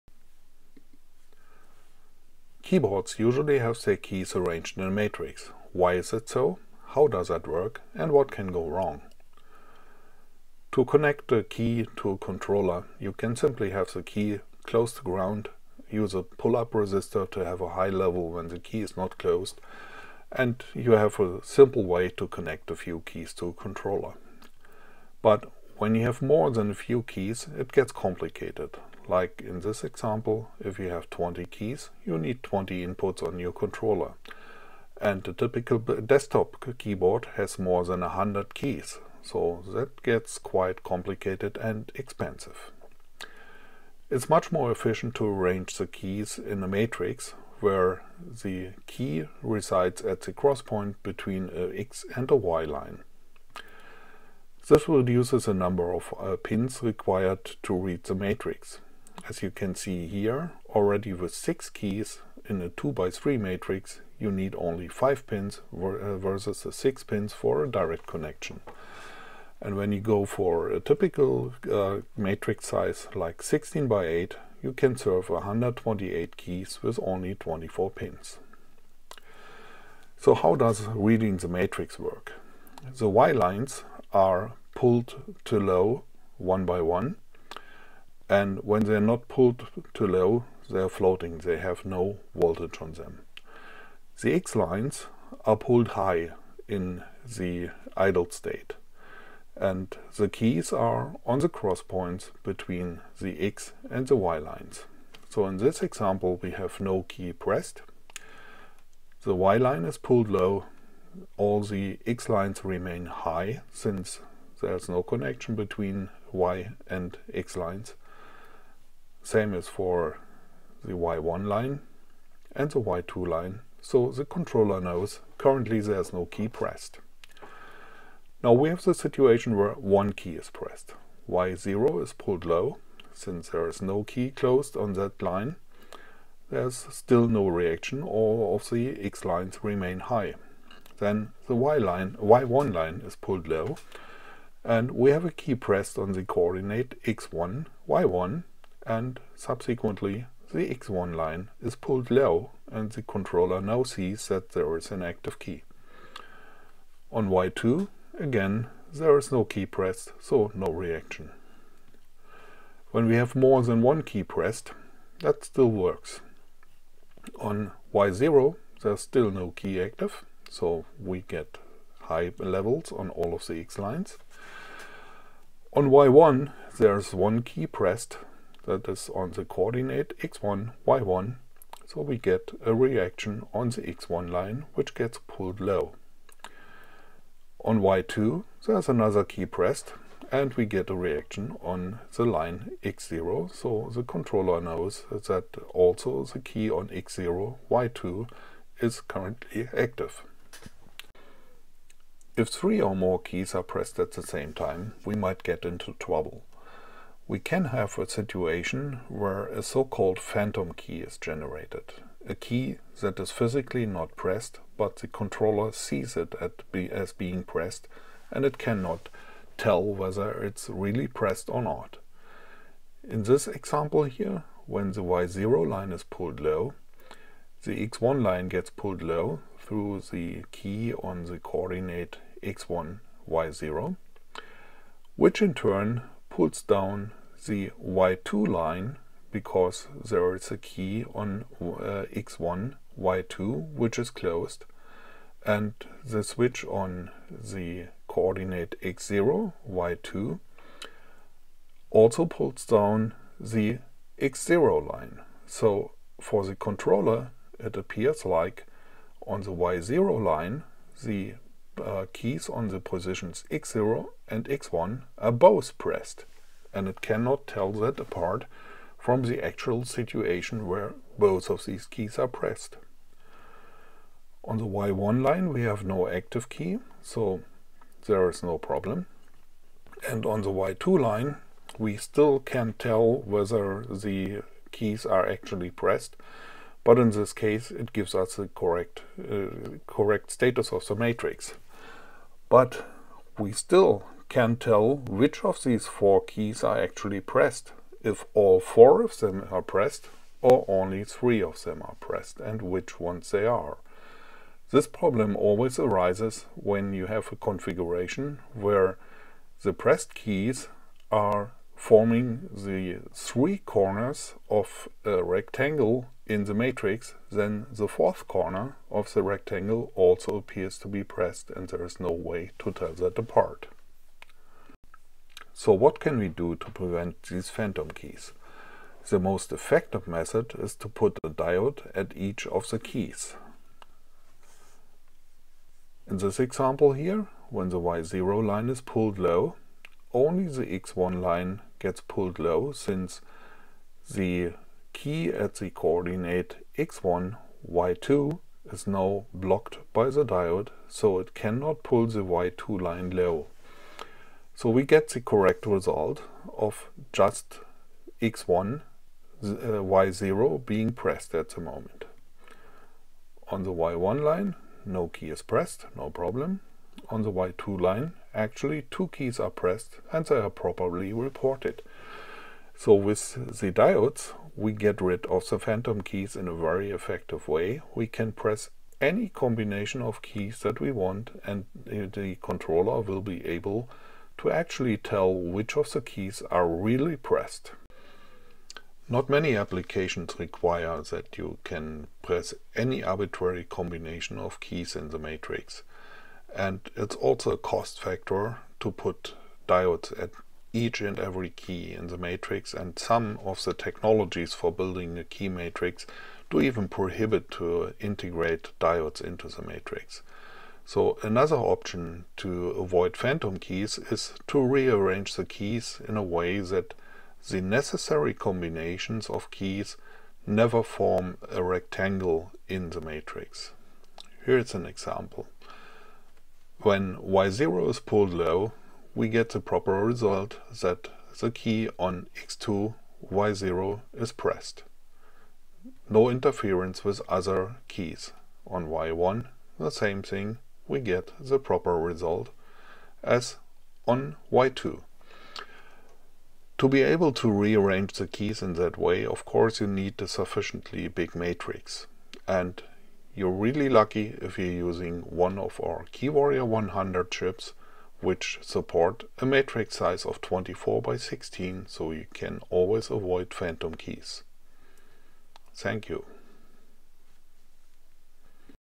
How does a keyboard matrix work? Why is it used? And what are phantom keys? And how to avoid them
A short explanation why keys on a keyboard are typically arranged as a matrix. How does a key matrix work and what are possible problems?
If not done properly a key matrix can result in phantom keys but there are ways to avoid that.

A good way to avoid phantom keys is to use a keyboard controller with a large matrix, like the KeyWarrior100: